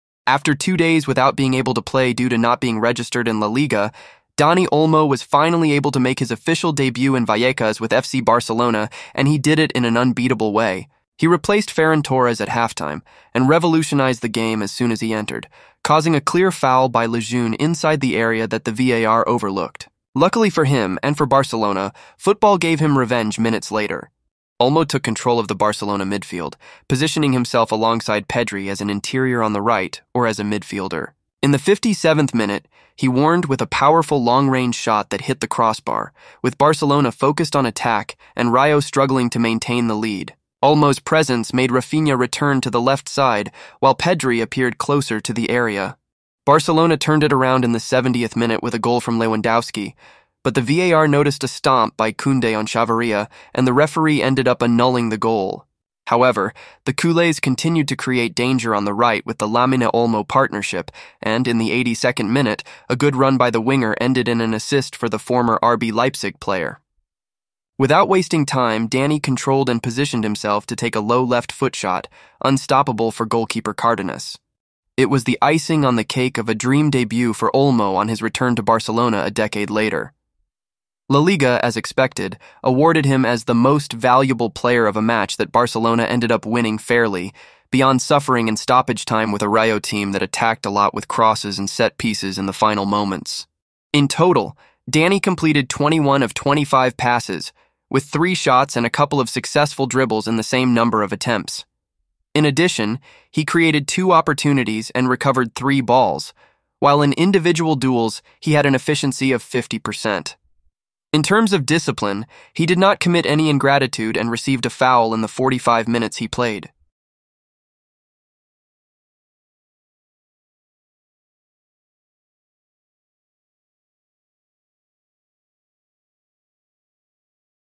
Lamine Yamal gives assist to Dani Olmo for a 2-1 win Against Rayo Vallecano | LALIGA
Dani Olmo scores winner on dream Barcelona debut in comeback win at Rayo Vallecano